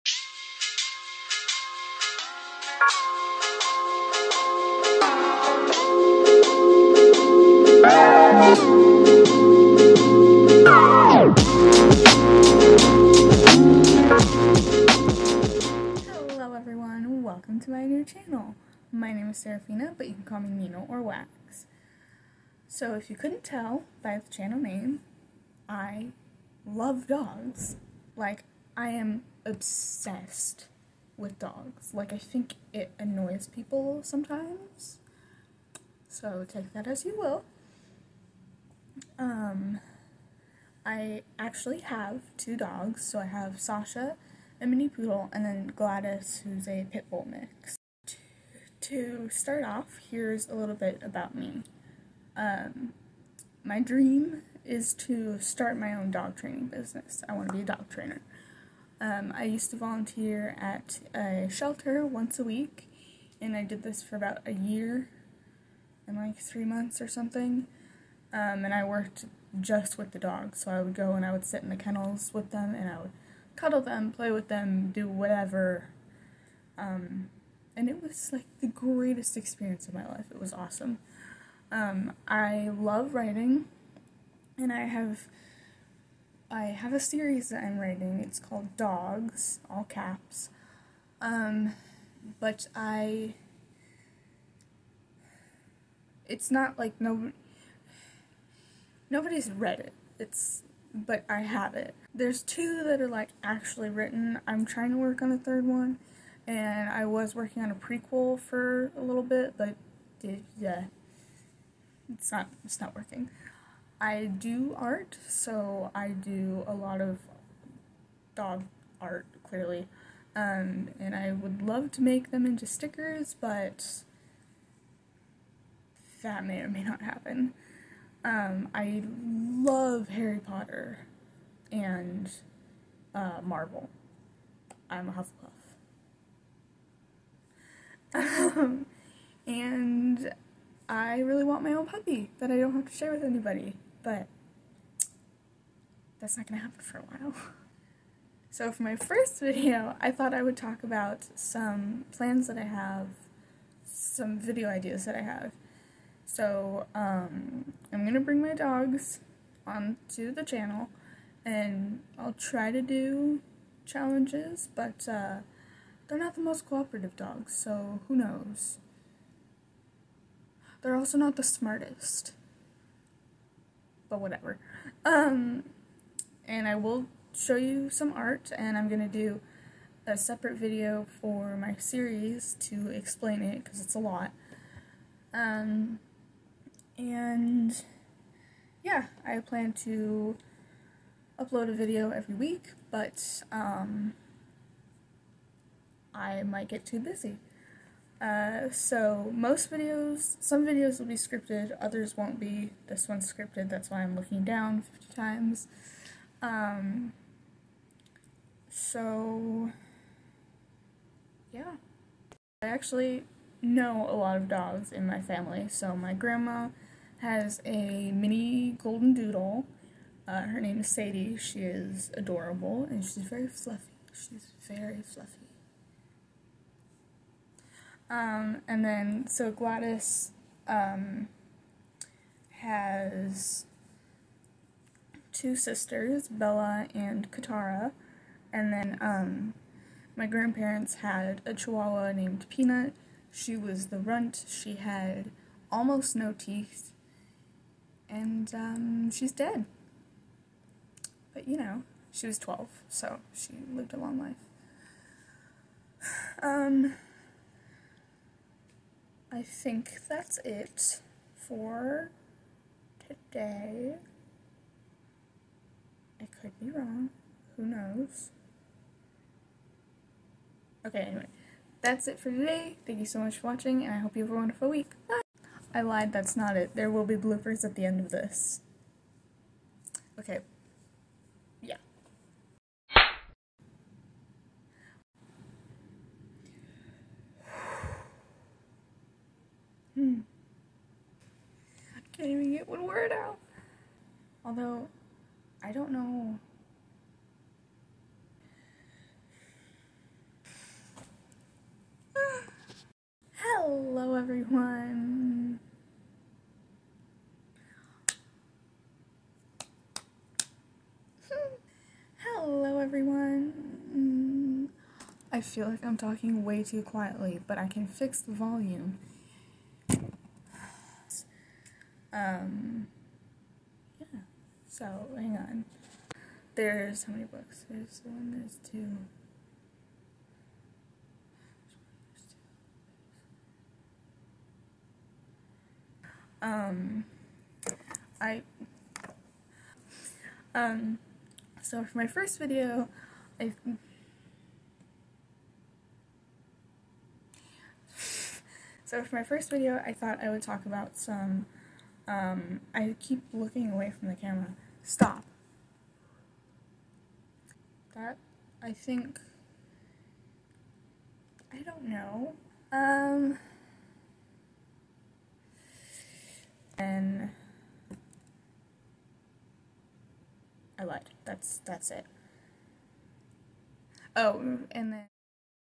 Welcome to the Wonderful World Of Wags!
Welcome to the Wonderful World Of Wags! My name is Seraphina, but you can call me Nina or Wags! Sorry, this video's a bit boring, but the rest will be better!